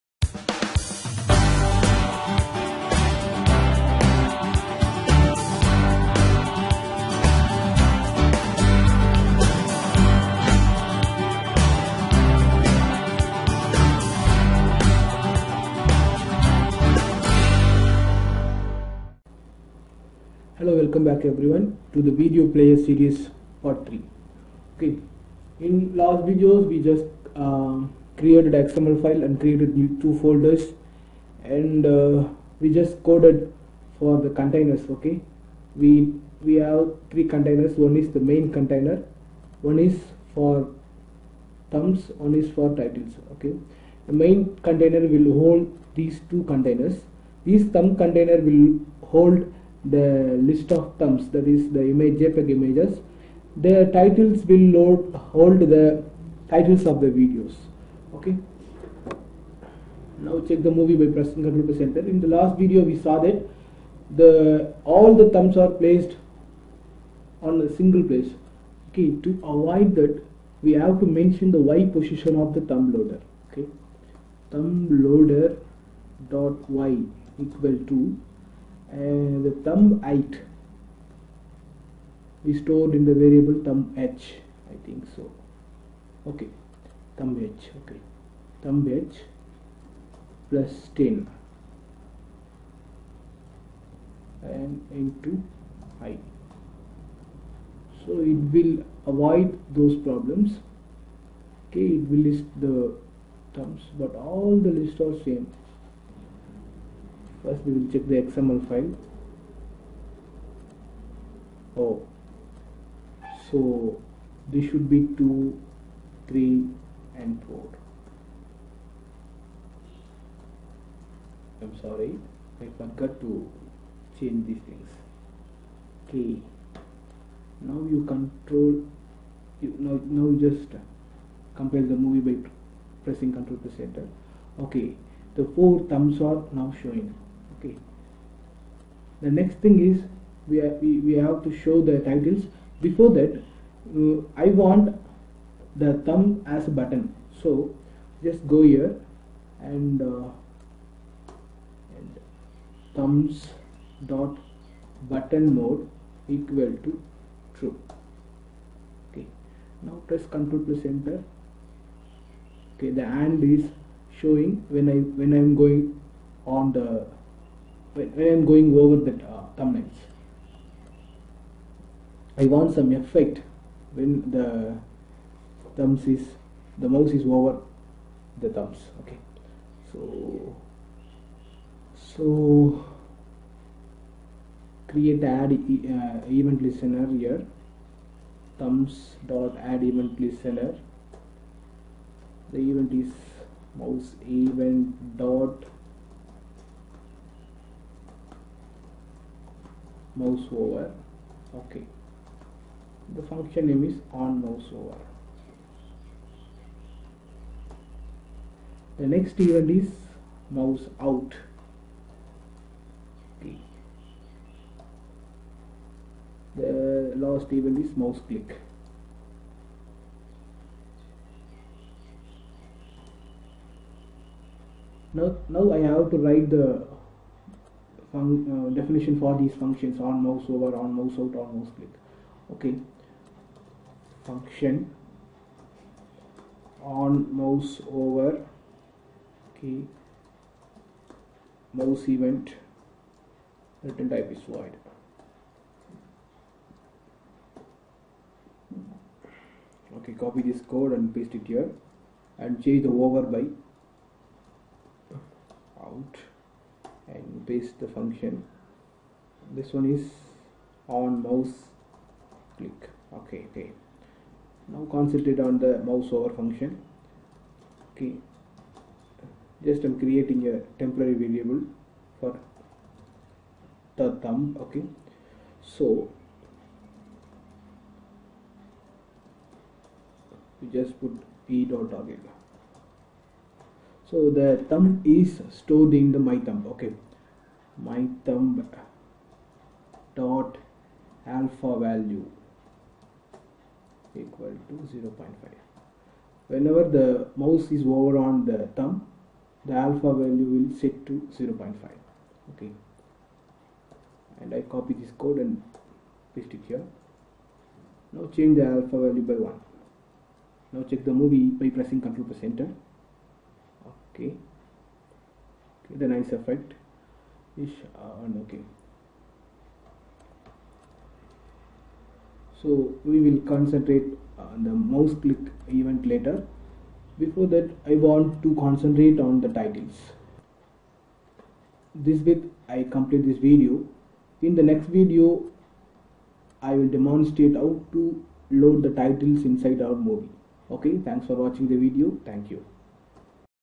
Flash video player using AS3.0 and XML Part 3/4 - Flash Tutorials
In this video tutorial, you can learn how to create a dynamic video player in flash as3.0 using XML.